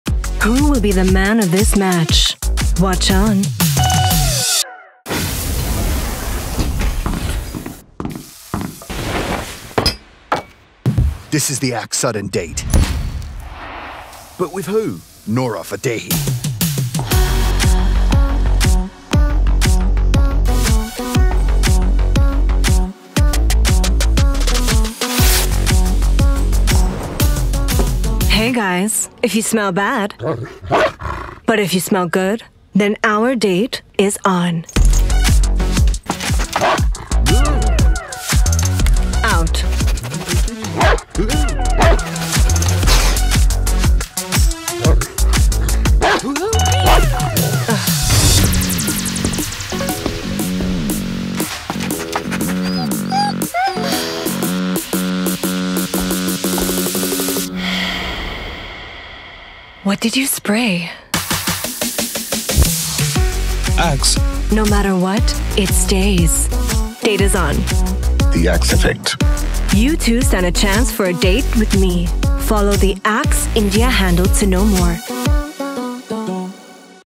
AXE SUDDEN DATE WITH NORA! - 1
TheNewAxeEffect is all about smelling amazing every moment in the day, and being forever ready for a sudden date, even if it’s with Queen Nora herself.

Watch-on how Axe keeps you repped for a sudden date, anytime, anywhere – literally; with their range of deodorants that make you smell attractive all day!

Log on to @axe_india on Instagram, to know if you too can be the lucky one for a sudden date with Nora.